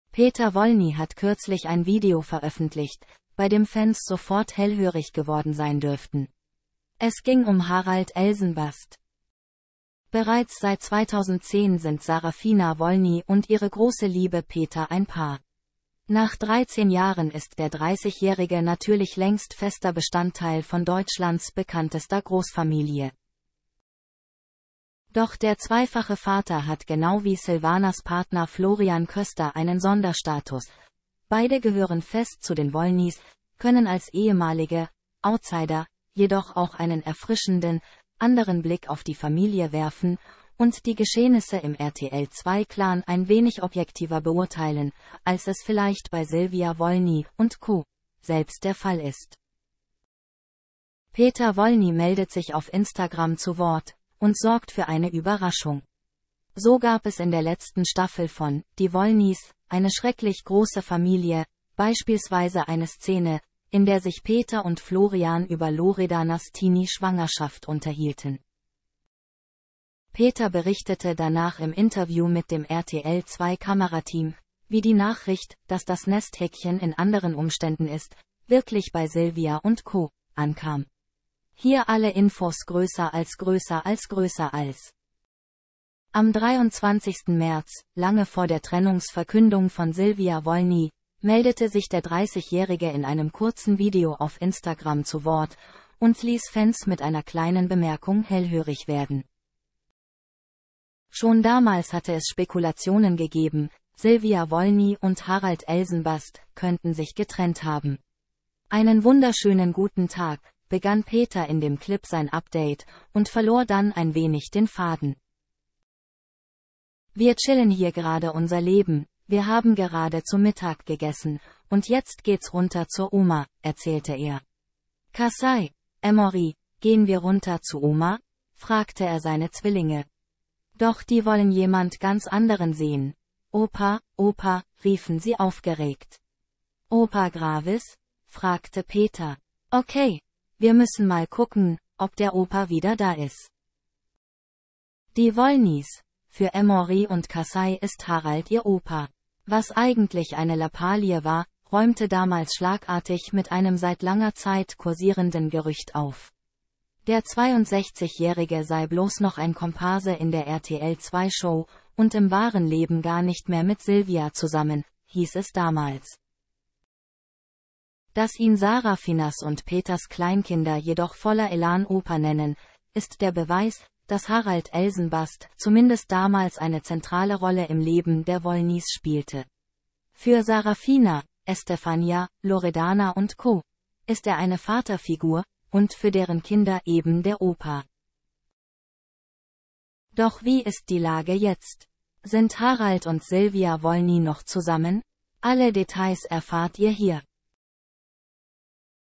Die Wollnys Emory & Casey plaudern Wahrheit über Harald und Silvia aus
Peter Wollny hat kürzlich ein Video veröffentlicht, bei dem Fans sofort hellhörig geworden sein dürften. Es ging um Harald Elsenbast!